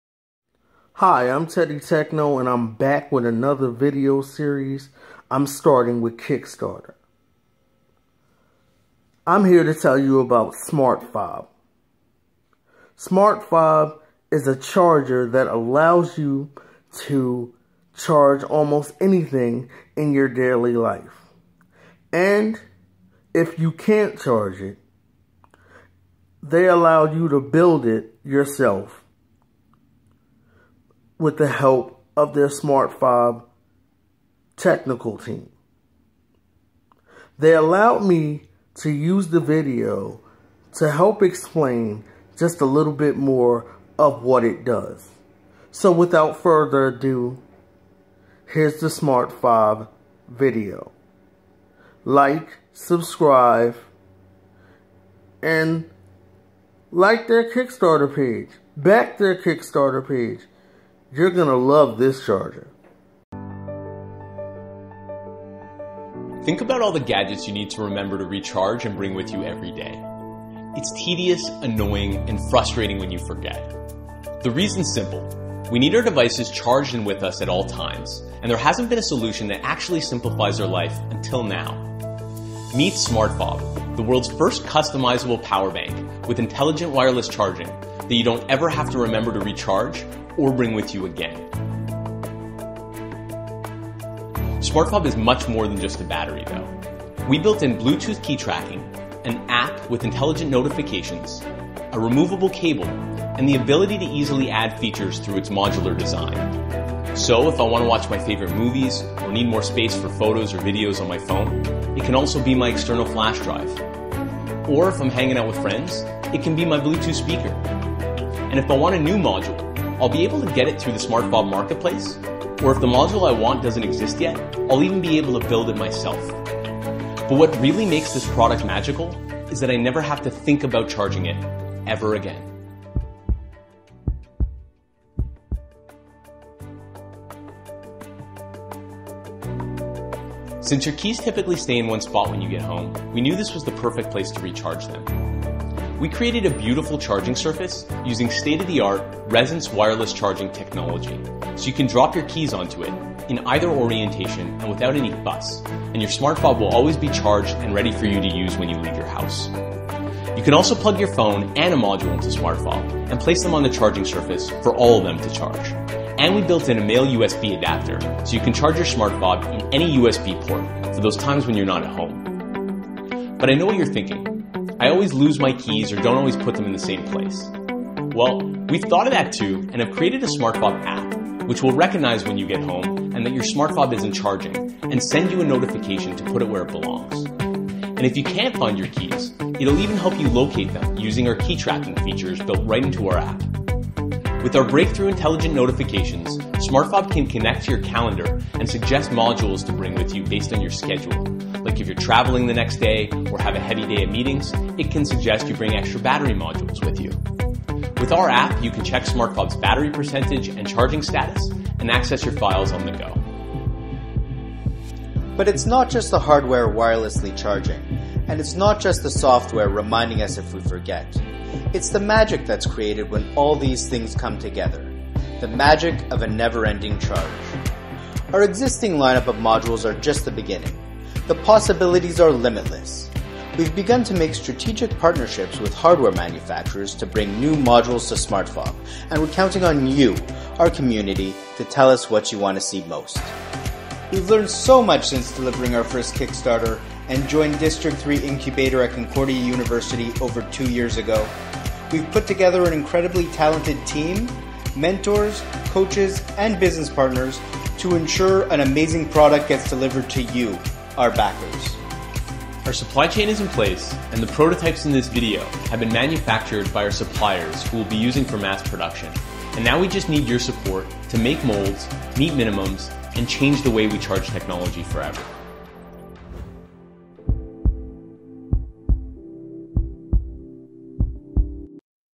SMARTFOB
World's MOST intelligent charger & PERSONAL-ASSISTANT app, charges wirelessly, reminds you to charge it and take it with you, & more!
smrtFOB: a smart-powerbank to charge your devices that fuses a modular design which allows you to customize its features, state-of-the-art wireless charging and an app with our breakthrough intelligent notifications. smrtFOB takes the thinking out of charging by reminding you to recharge it and bring it with you if you forget
You've probably got a lot of important things to remember in a day and we think remembering to charge your devices shouldn't be one of them. We created a smrtFOB app, which will recognize when you get home (or another preset location) and that your smrtFOB isn’t charging and send you a reminder to charge it